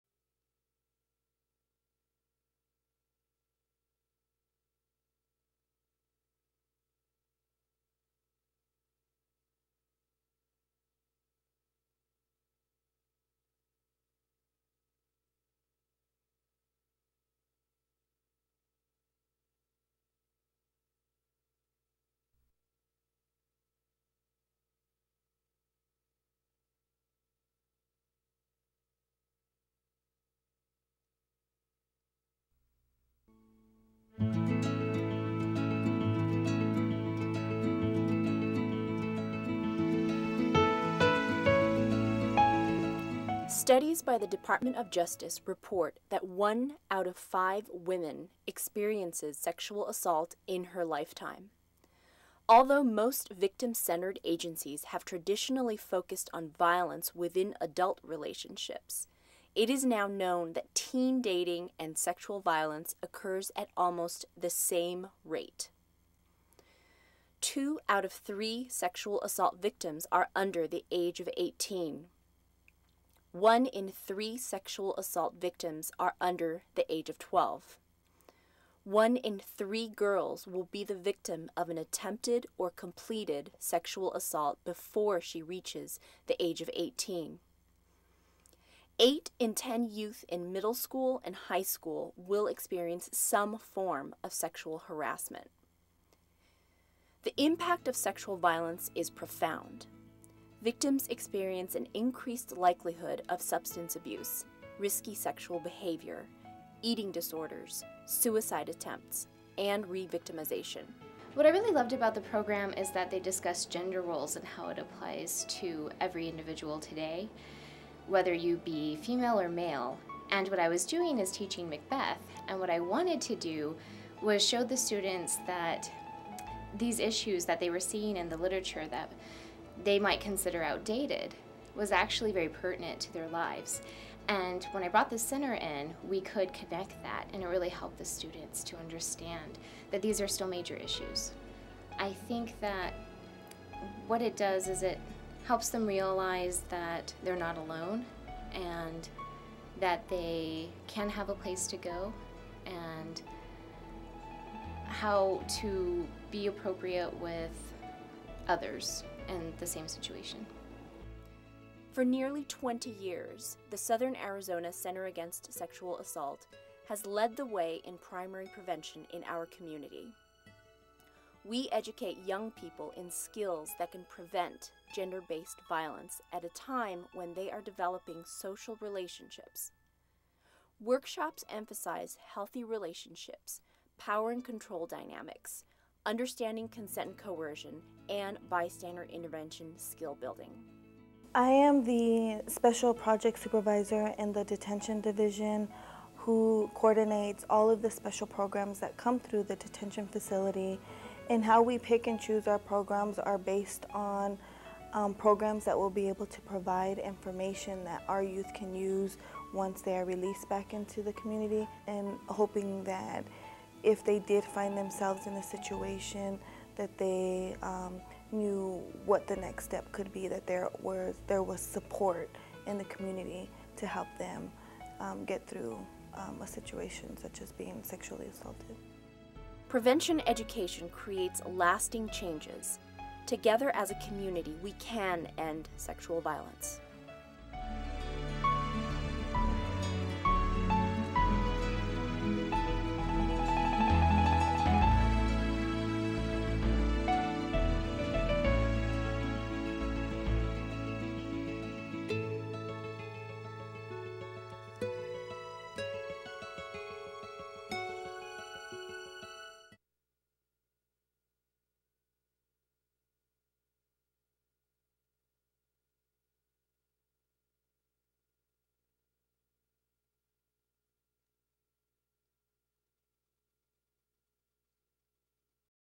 Southern Arizona Center Against Sexual Assault
The Southern Arizona Center Against Sexual Assault's mission to reduce the trauma and incidence of sexual assault by providing treatment and promoting prevention of sexual abuse, incest, molestation and rape.